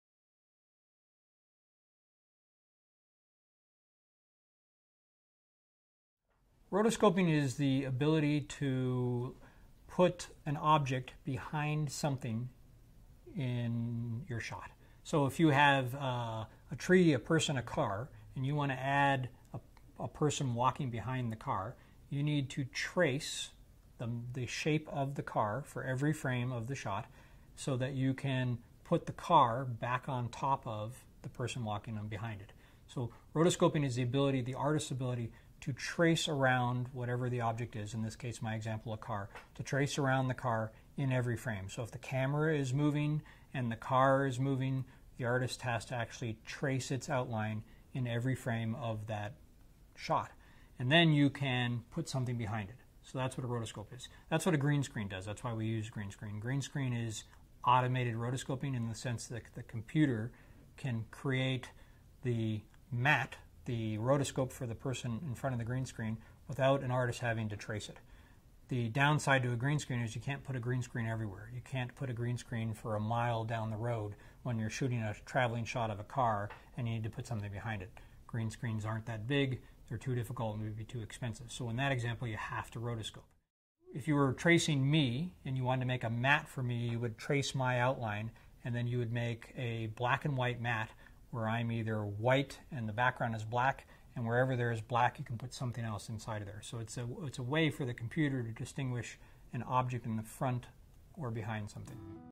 "Roto Scoping" what is it and is it expensive? - VFX basicfilm - w Raymond McIntyre
Looking at"Roto Scoping"  What can it cost?

basicfilm - POST w Raymond McIntyre ep.26